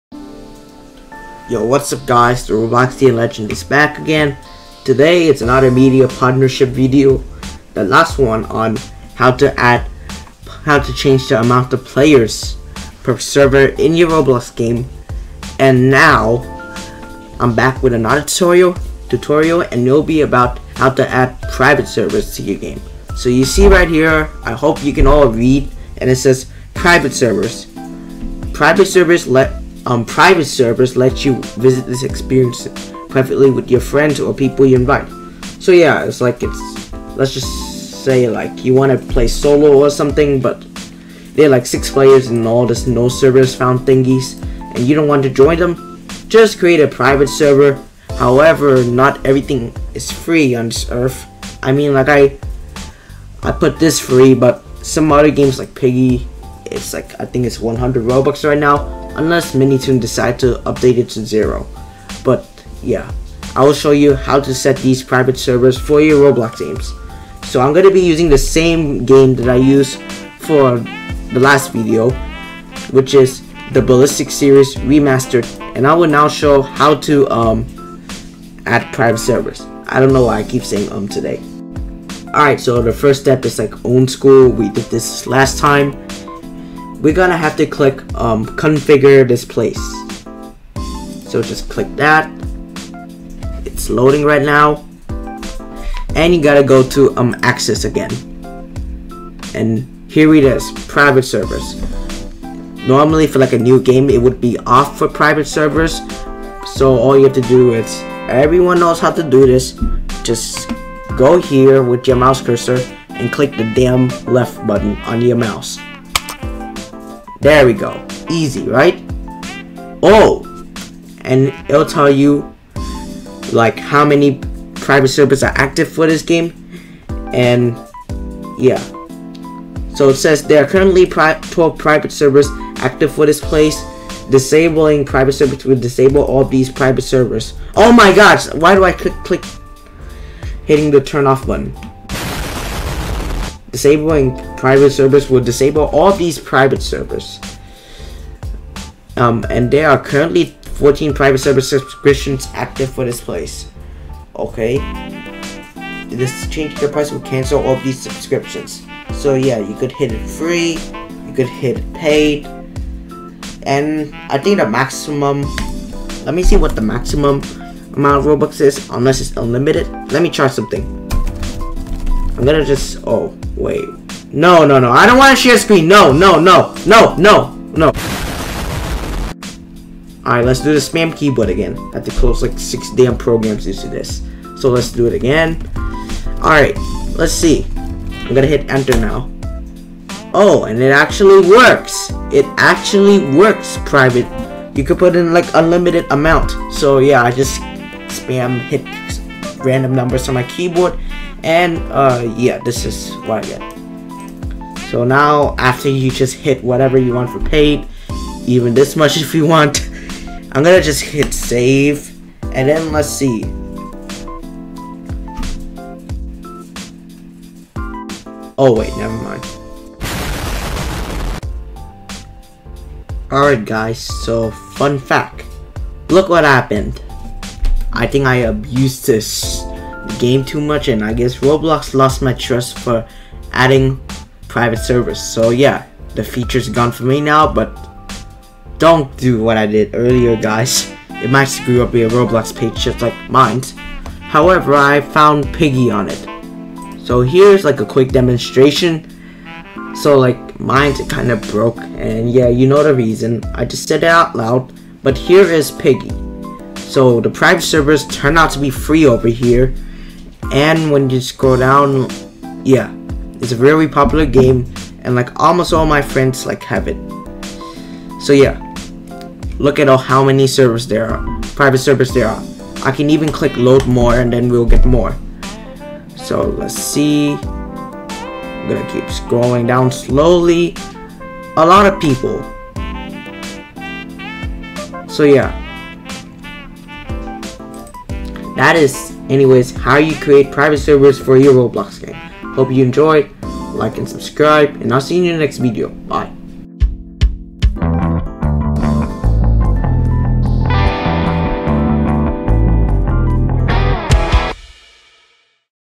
How to Add Private Servers to Your Own Roblox Games (Media Partner Video)
Today, THE ROBLOXIAN LEGEND will show us how to add private servers to our own Roblox games!